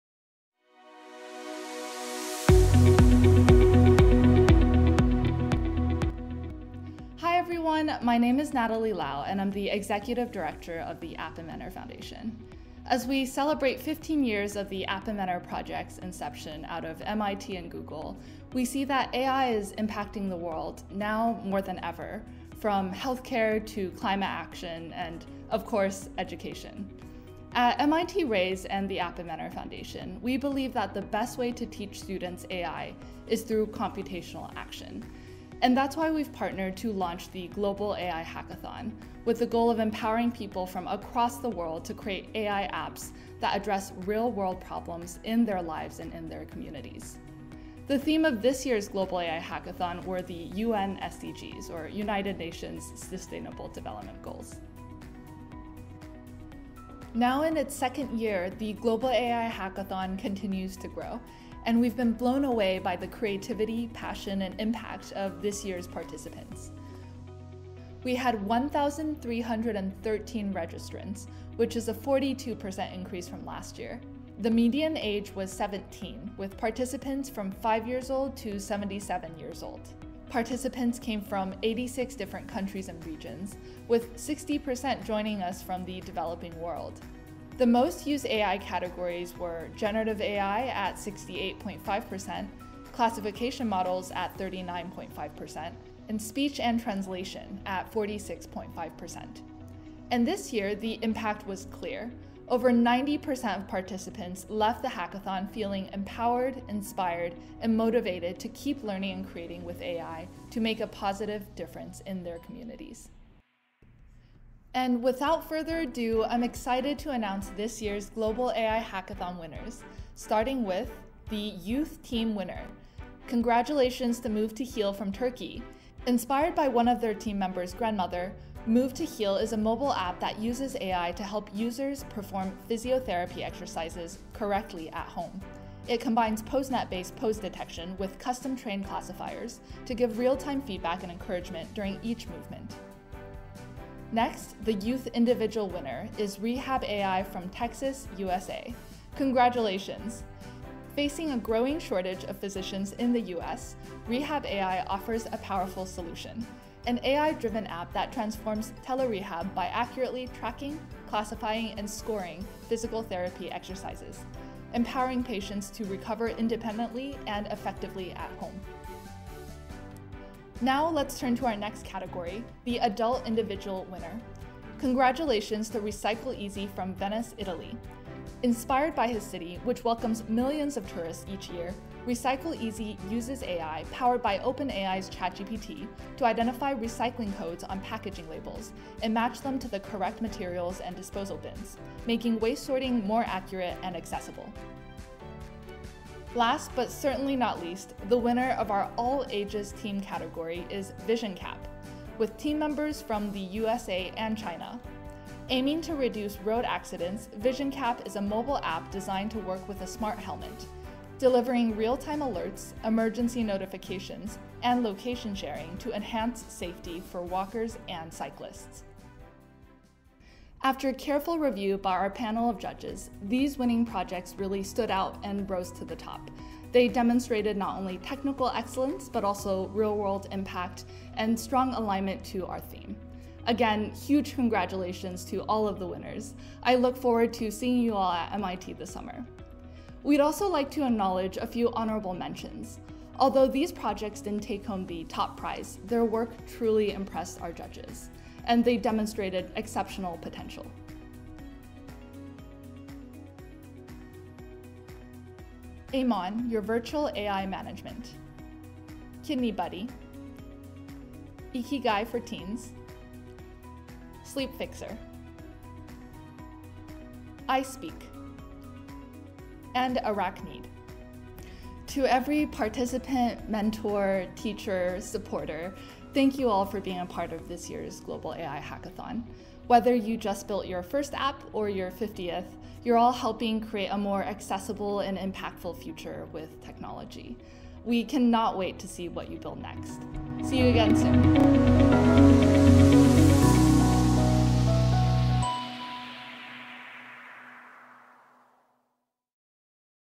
Global AI Hackathon 2025 Winners Announcement [Updated]
We're thrilled to share the results of our second annual Global AI Hackathon, which brought together 1,313 participants from 86 countries and regions to develop AI-powered mobile applications inspired by the UN SDGs (Sustainable Development Goals). Over the six-week competition, participants—who ranged from 5 to 77 years old—created impressive projects spanning all seventeen UN SDGs, with healthcare and education solutions being the most popular topics. Meet the talented winners and honorable mentions in this video!